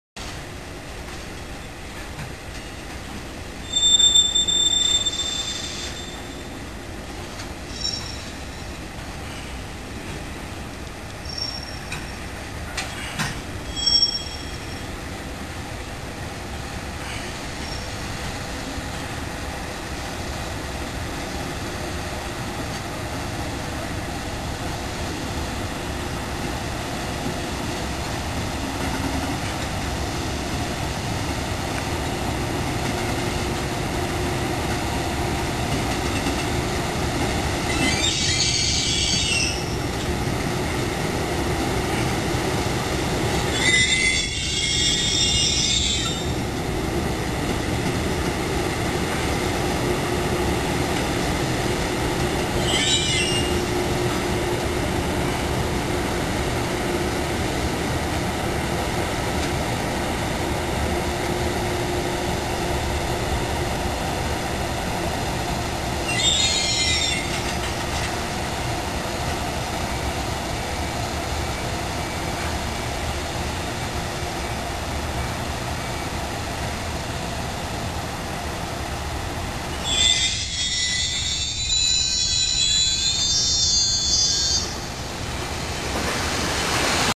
「いっしょに青森」ステッカーを貼ったJR貨物の機関車＠岩沼駅
東日本大震災からの復興を応援する「がんばろう、にっぽん　いっしょに青森」「がんばろう東北」ステッカーを貼ったEH500金太郎が、コンテナを積んだ貨車をバックで牽引するところです。
（平成23年8月18日、岩沼駅で撮影）